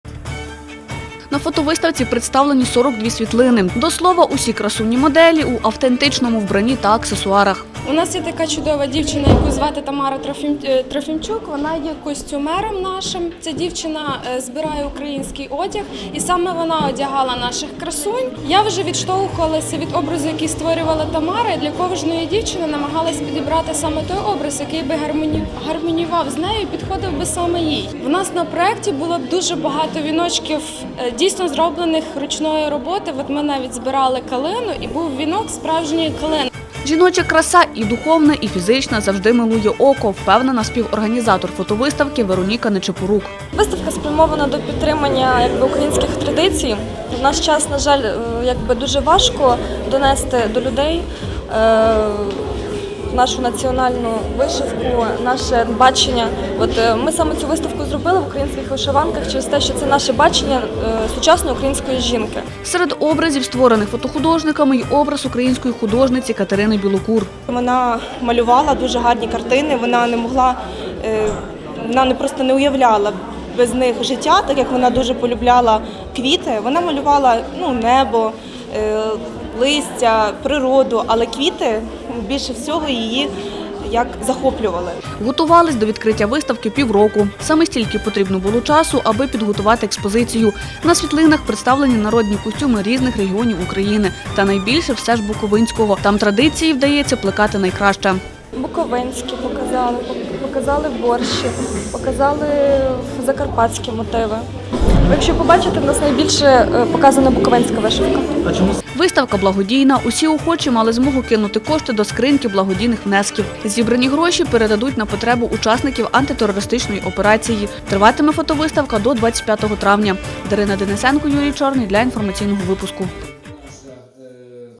В обласному центрі презентували фотовиставку Українська краса
В обласному центрі презентували фотовиставку «Українська краса – не дай себе забути». Мета  експозиції – показати красу й української жінки, і національного одягу.